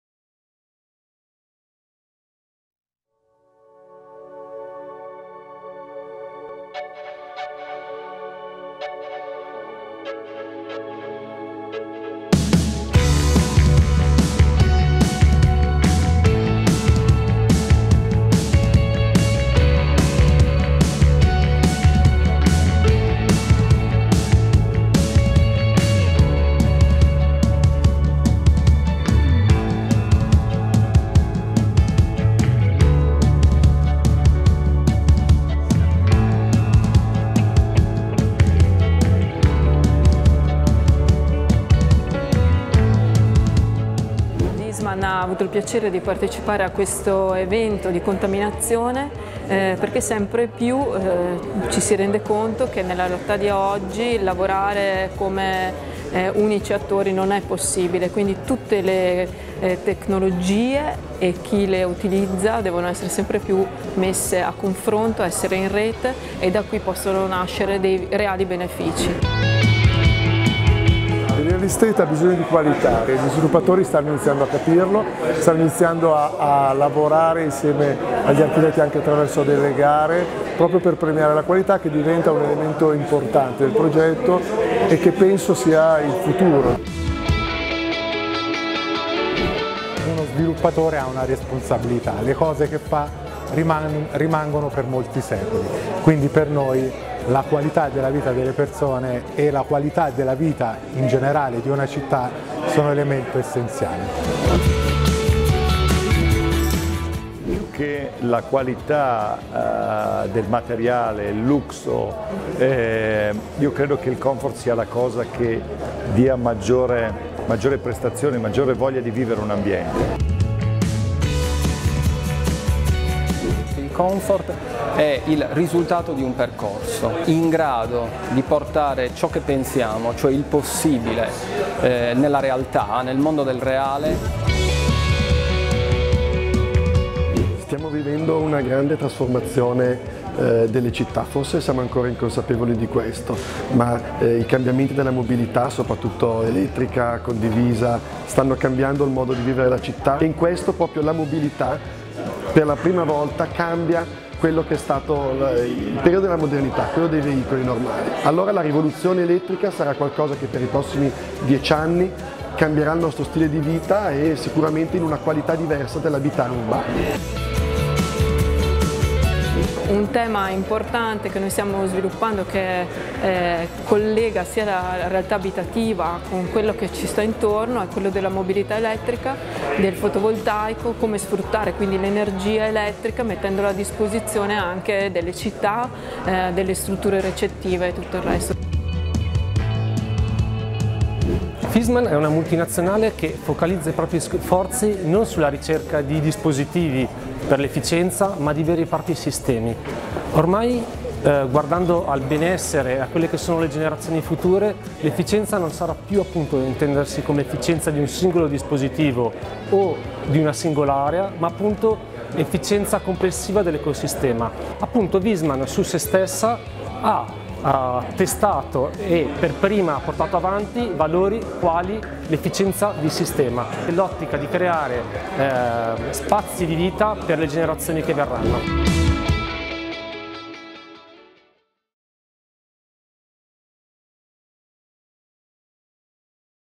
Ricette d'Architettura | Viessmann al Fuorisalone 2019
Viessmann, in occasione del Fuorisalone 2019, è stato main sponsor dello show cooking Ricette d’Architettura, organizzato da PPAN nel centro City ZEN a Milano. Un evento incentrato sui temi dell’innovazione e della sostenibilità nella filiera delle costruzioni e sulla creazione di nuove relazioni, al fine di generare possibili contaminazioni di professioni e competenze. Guarda il video e le interviste!